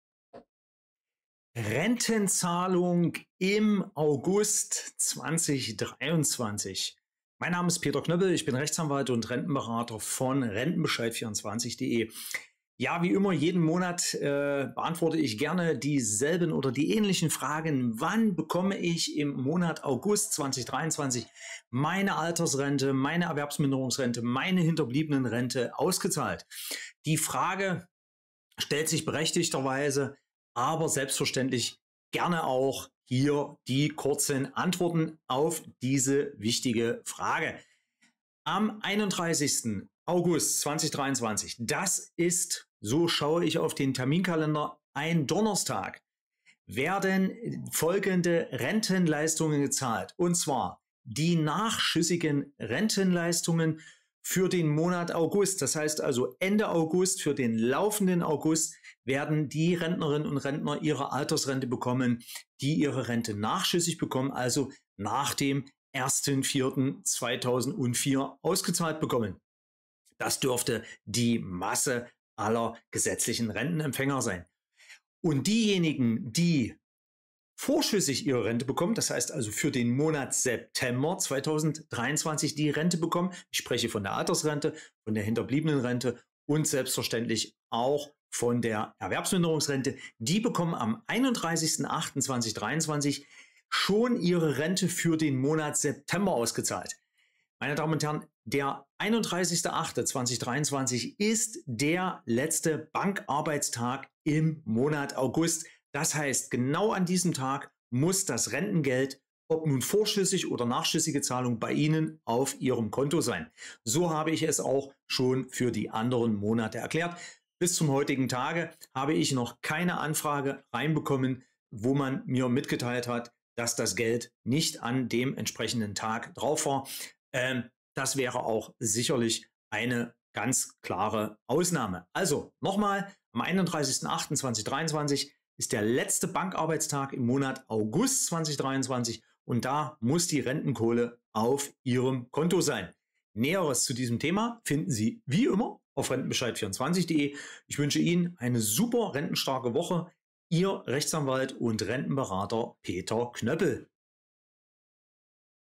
Rentenzahlung August 2023: Wann wird das Rentengeld gezahlt?
Wann wird die Rente im Monat August 2023 gezahlt. Am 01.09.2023 einen Freitag oder den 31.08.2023 den Donnerstag, als letzten Bankarbeitstag. Unser Rentenberater Peter Knöppel erläutert Ihnen, wann die Rente im Monat August 2023 gezahlt wird. Stichwort vorschüssige oder nachschüssige Rentenzahlung!